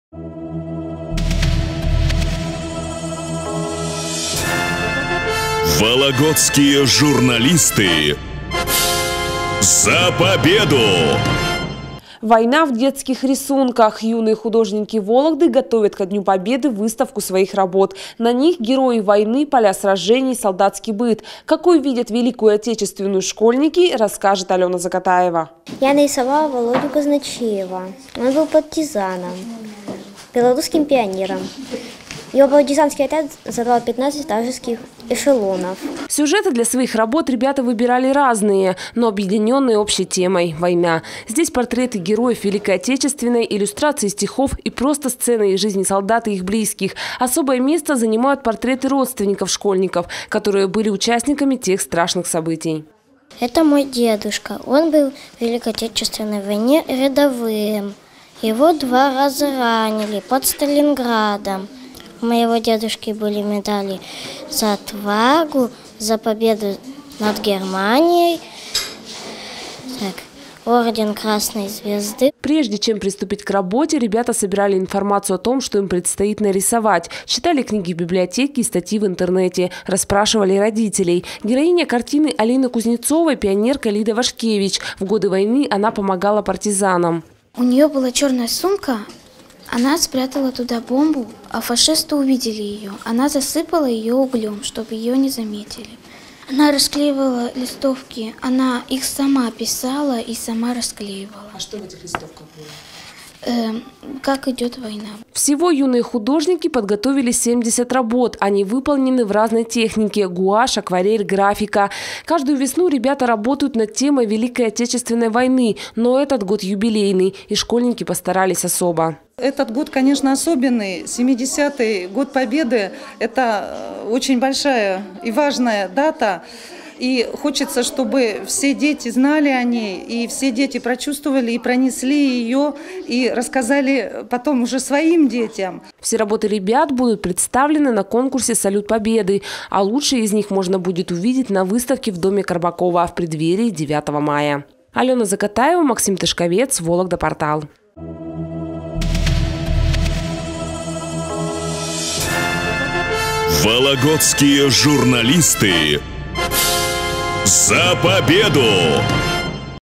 Юные художники Вологды готовят к юбилею Победы выставку своих работ
Сюжеты для своих работ ребята выбирали разные, но объединенные общей темой – война. Это портреты героев Великой Отечественной войны, иллюстрации к стихам и просто сцены из жизни солдат и их близких. Особое место занимают портреты прапрадедов школьников, которые были участниками тех страшных событий.